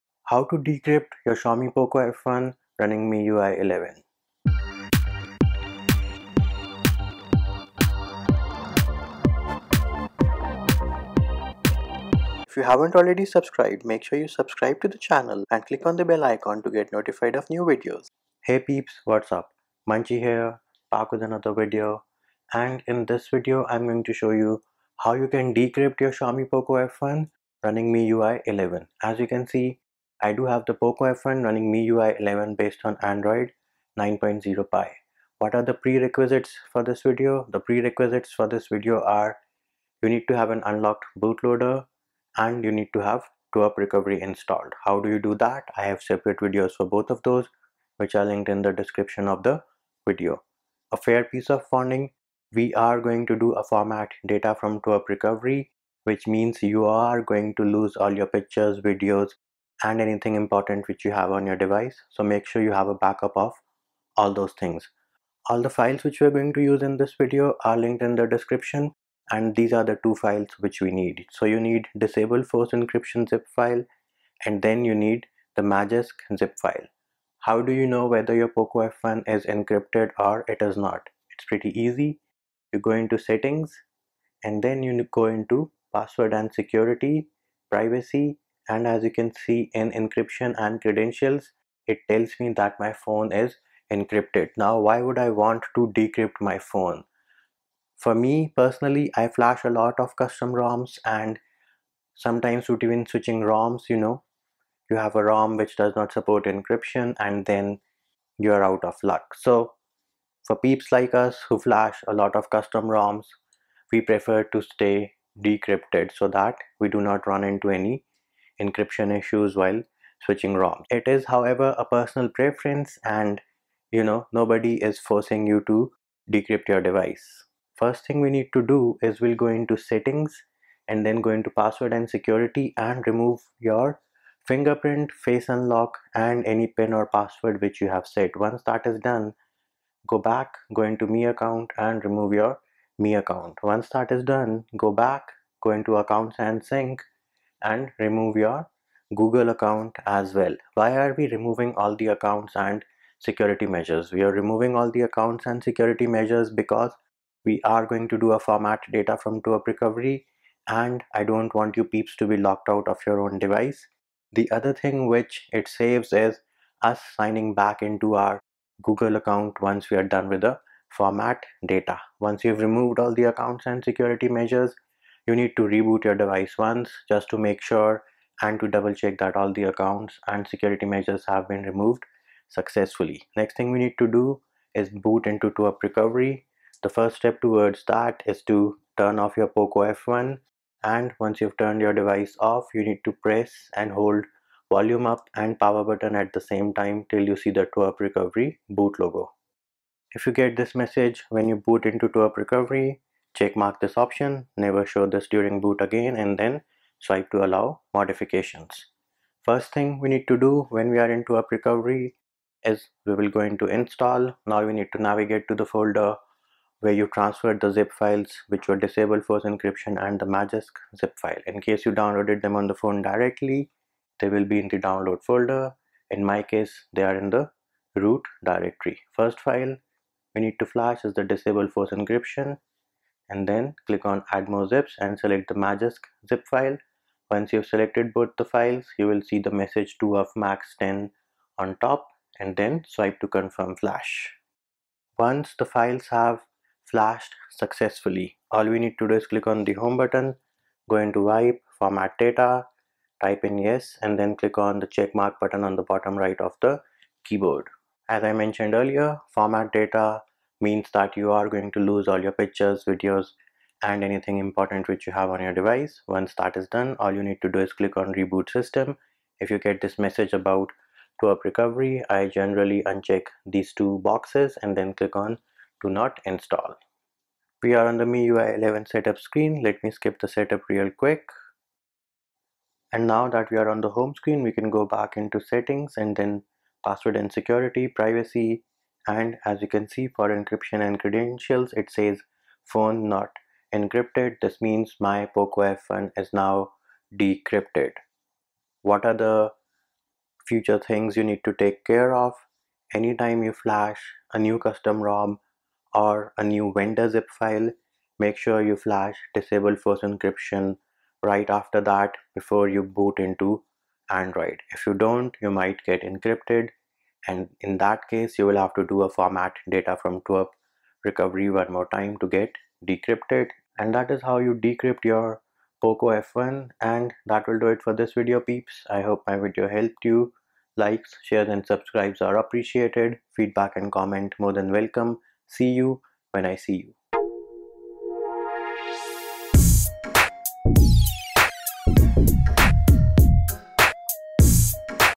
Xiaomi Poco F1 | How to Remove Encryption | How to Decrypt | TWRP
Do you want to learn how to remove encryption or decrypt from your Xiaomi Poco F1 running MIUI 11? In this detailed video tutorial we show you how to remove encryption or decrypt from your Xiaomi Poco F1 running MIUI 11. You will need to have TWRP Recovery installed on your Xiaomi Poco F1.